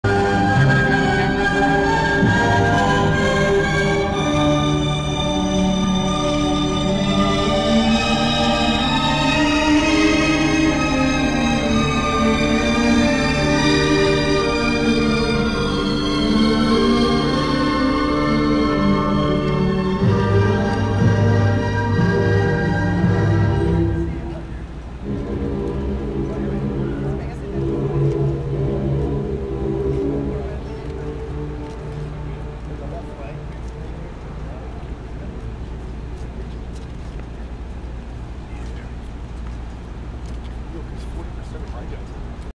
AZ: The McCain Truth Squad Procession
To start the rally, the Truth Squad had a funeral procession through the parking lot where McCain's main offices are.